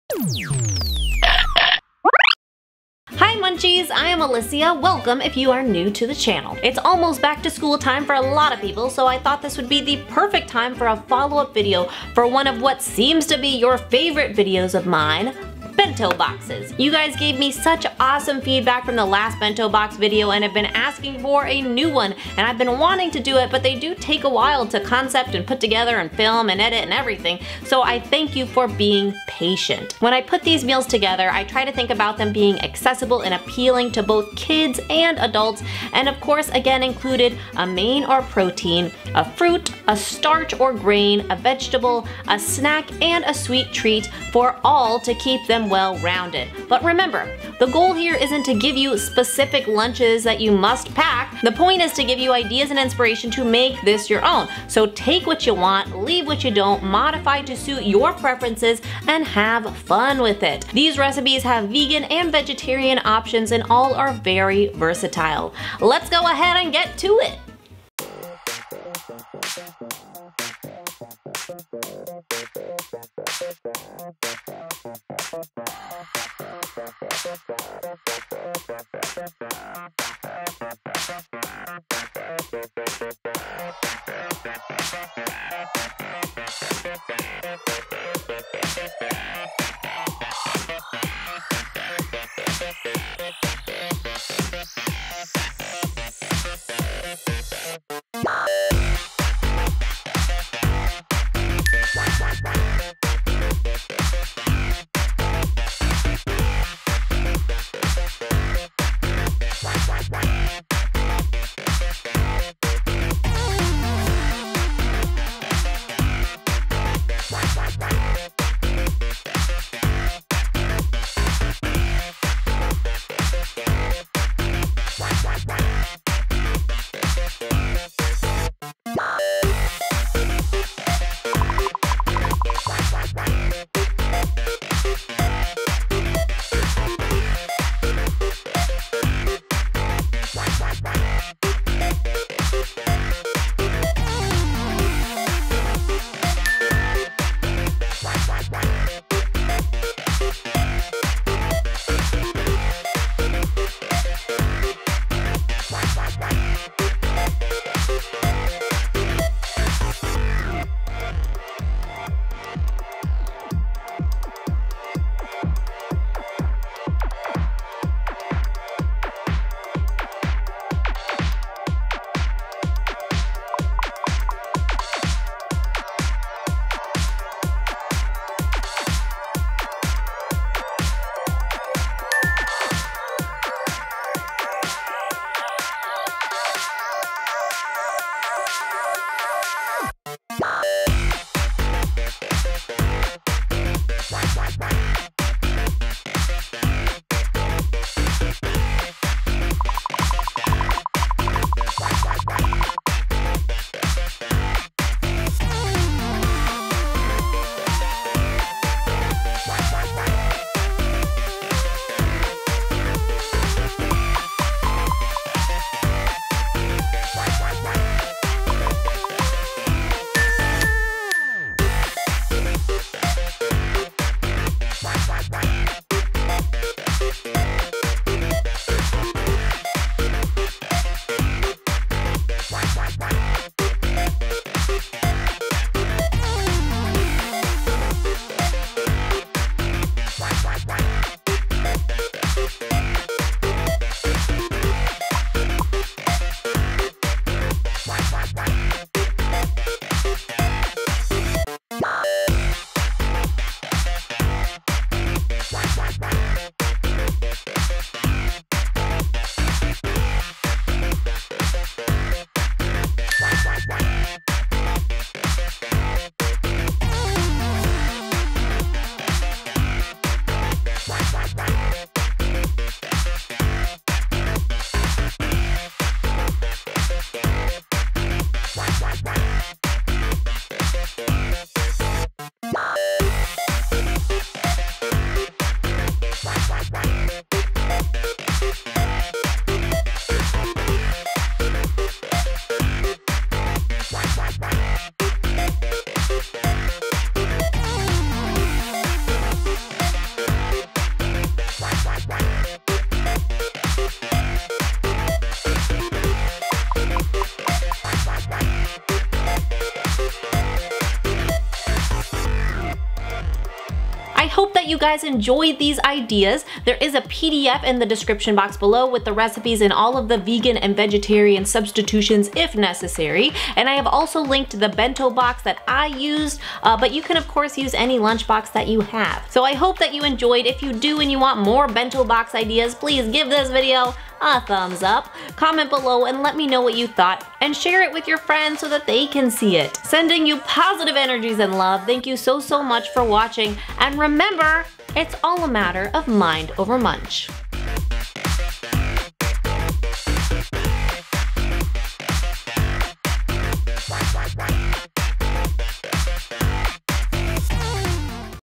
Bento Box Lunches | Healthy Recipes! - Mind over Munch
MORE Bento Box lunch ideas! Healthy recipes like sweet potato chips, vegan rice krispies, spring rolls, sushi sandwiches, & more! These lunch ideas are just base recipes, so get creative and adapt them to suit your lifestyle!

STUFF From this video:
► Bento Box
► Silicone Molds
► Cookie Cutters for Veggies
► Cookie Cutter for Uncrustable
► Veggie Spiralizer
► Spring Roll Wrappers